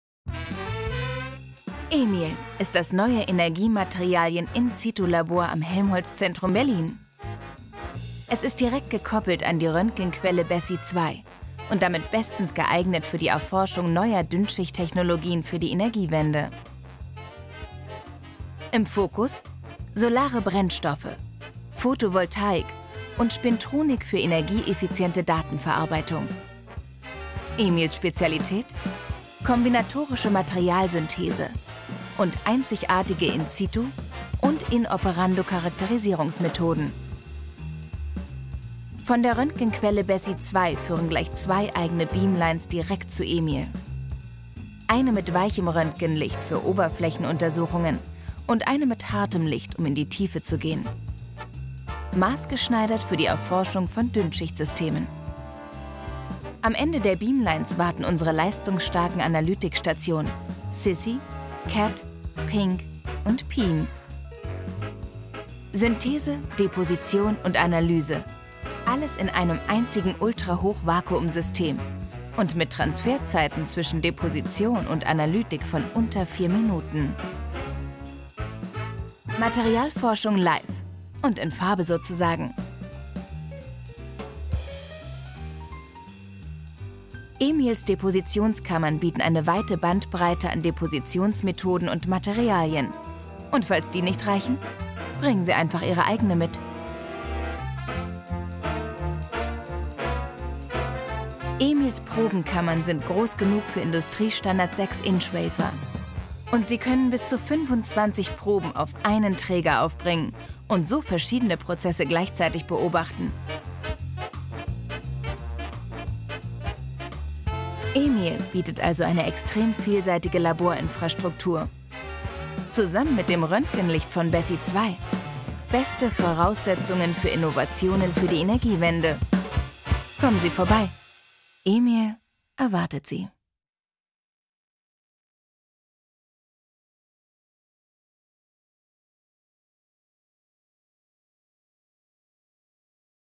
EMIL - Energy Materials In-Situ Lab (DE)
Auftraggeber: Helmholtz-Zentrum Berlin

"Emil und die Detektive" ist ein bekanntes Kinderbuch. Unser EMIL ist auch eine Art Detektiv. Hier gehen Forscher auf die Suche nach Defekten im Kristallgitter von Solarzellen. Sie ermitteln, wie Defekte den Wirkungsgrad beeinflussen. Und zwar nicht nur in Solarzellen, sondern generell in Energiematerialien. EMIL steht deshalb für Energy Materials In-situ Laboratory. Es wird gemeinsam vom HZB und der Max-Planck-Gesellschaft betrieben. Das Labor ist direkt angeschlossen an das Synchrotron BESSY II und bezieht von dort das spezielle Röntgenlicht, mit dem die Detektive arbeiten.